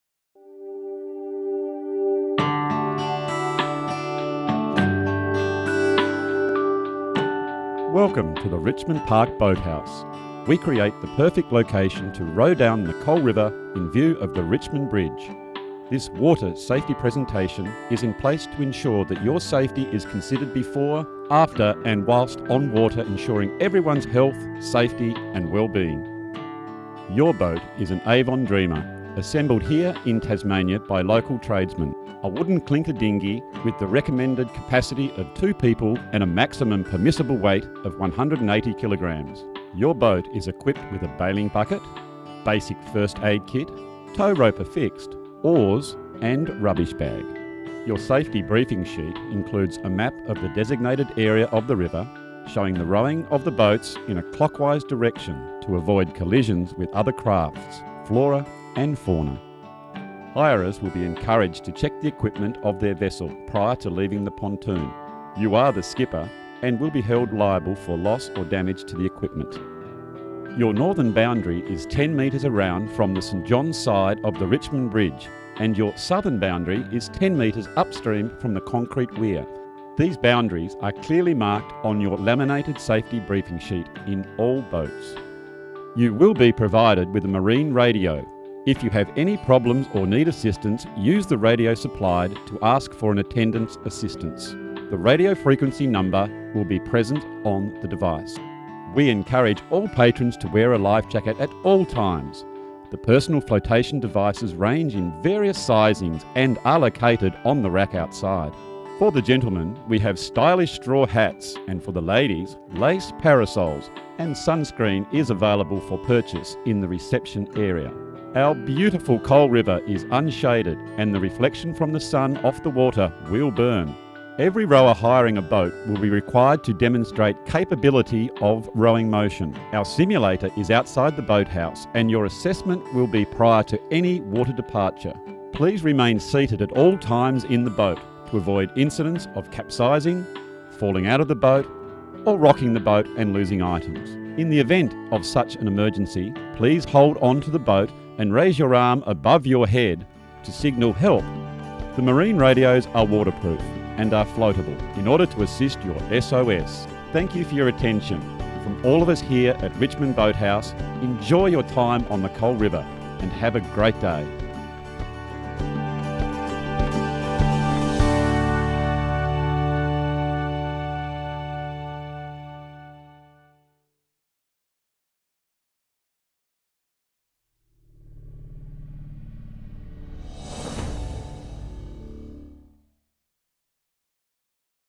Richmond Boathouse Safety video (30.10.2012)
3 minute video explaining safety & awareness when rowing a boat hired from the Richmond Boathouse, Richmond Tasmania